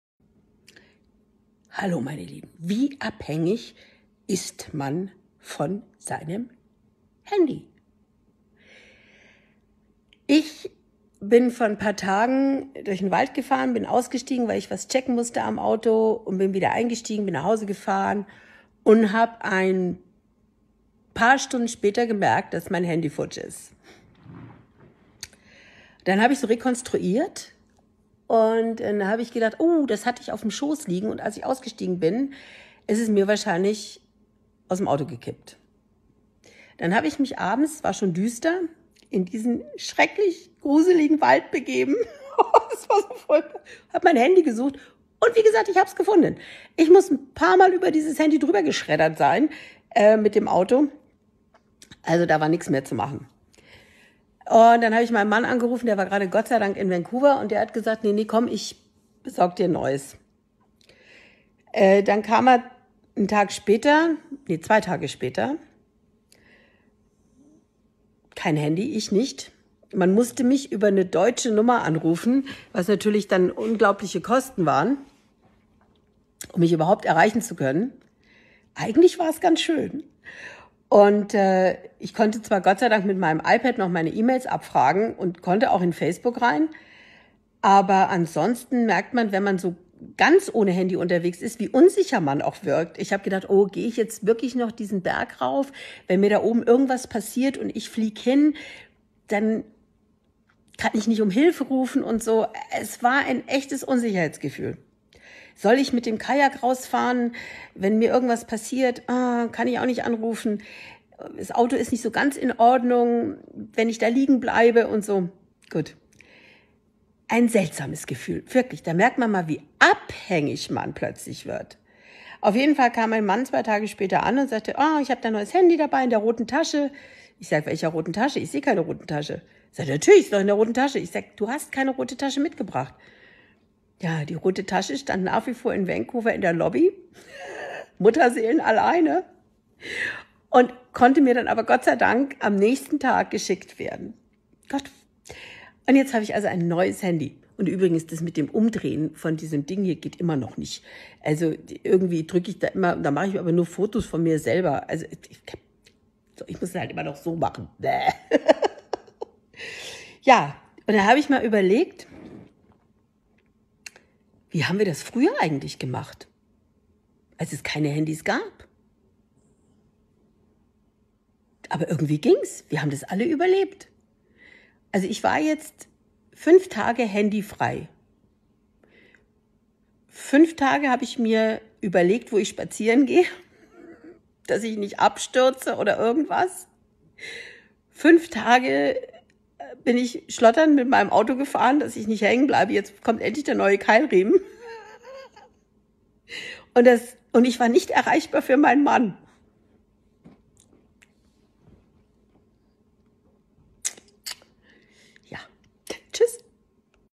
-55- Abhängigkeit vom Handy
KaterinaJacob über die Zerstörung ihres Handys und der gefühlten Abhängigkeit und den Komplikation der Erlangung eines neuen Handys.